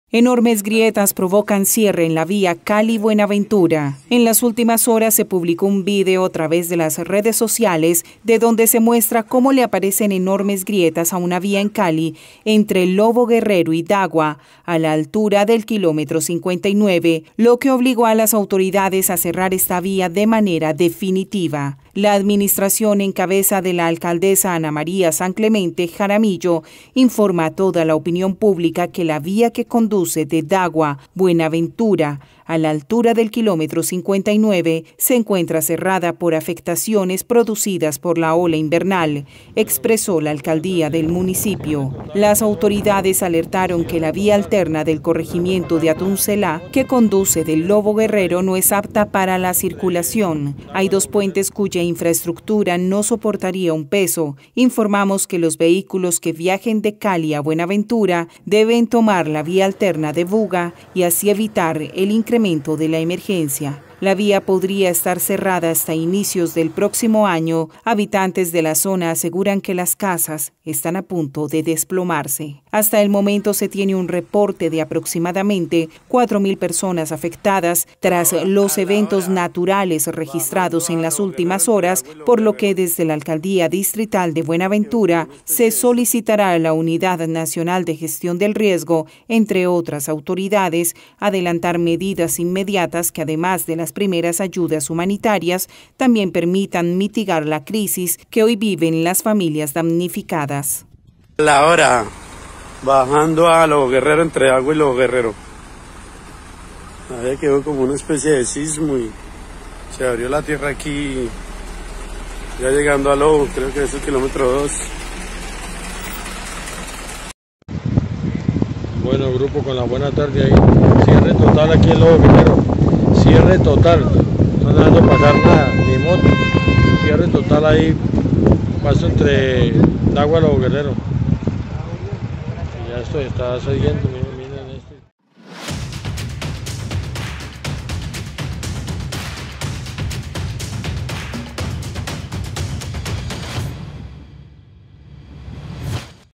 Mira Como Se Abre La Tierra en Cali y Destruye Toda La Carretera, Mas De 4.000 Personas en Alerta
¡Saludos a todos y GRACIAS por ese grandioso apoyo que nos brindan!

SIGUE A ''ULTRAMIX TV''  EN FACEBOOK
.IGNORAR.